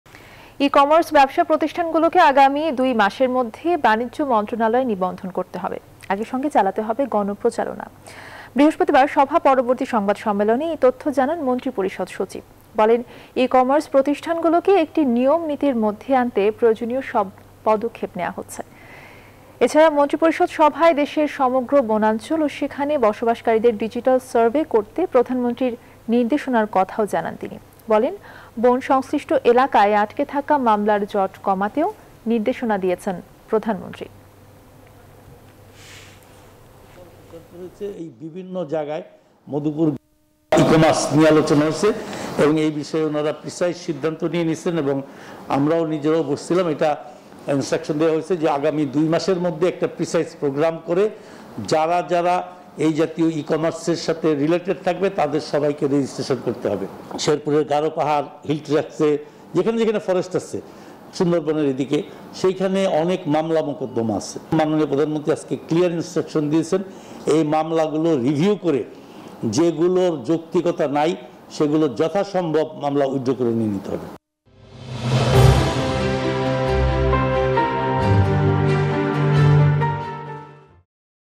সব ই-কমার্স প্রতিষ্ঠানকে নিয়ম নীতির মধ্যে আনা হচ্ছে 28Oct.21
ই-কমার্স ব্যবসা প্রতিষ্ঠানগুলোকে আগামী ২ মাসের মধ্যে বাণিজ্য মন্ত্রণালয়ে নিবন্ধন করতে হবে। একইসঙ্গে চালাতে হবে গণপ্রচারণা।

বৃহস্পতিবার সভা পরবর্তী সংবাদ সম্মেলনে এই তথ্য জানান মন্ত্রীপরিষদ সচিব। বলেন, ই-কমার্স প্রতিষ্ঠানগুলোকে একটি নিয়মনীতির মধ্যে আনতে প্রয়োজনীয় সব পদক্ষেপ নেয়া হচ্ছে। এছাড়া মন্ত্রীপরিষদ সভায়, দেশের সমগ্র বনাঞ্চল ও সেখানে বসবাসকারিদের ডিজিটাল সার্ভে করতে প্রধানমন্ত্রীর নির্দেশনার কথাও জানান তিনি। বলেন, বন সংশ্লিষ্ট এলাকায় আটকে থাকা মামলার জট কমাতেও নির্দেশনা দিয়েছেন প্রধানমন্ত্রী।

Our other YouTube channels :
News24 Official Address:
NEWS24
371/A
Bashundhara Road
Block – D
Bashundhara Residential Area
Dhaka - 1229